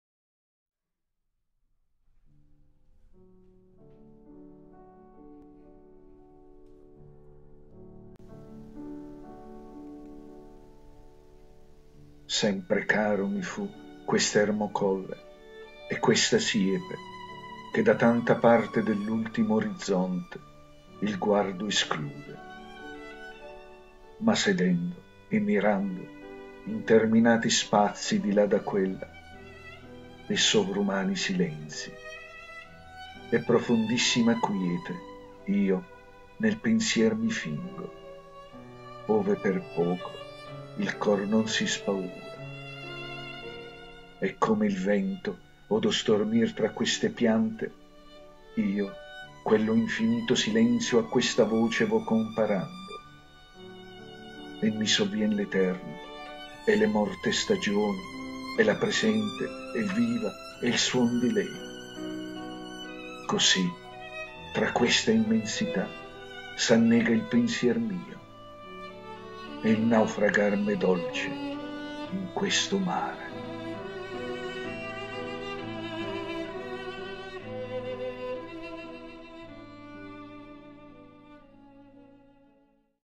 Giacomo Leopardi. "L'Infinito". Voce di Alfred B. Revenge
"L'Infinito" poesia di Giacomo Leopardi. Voce di Alfred B. Revenge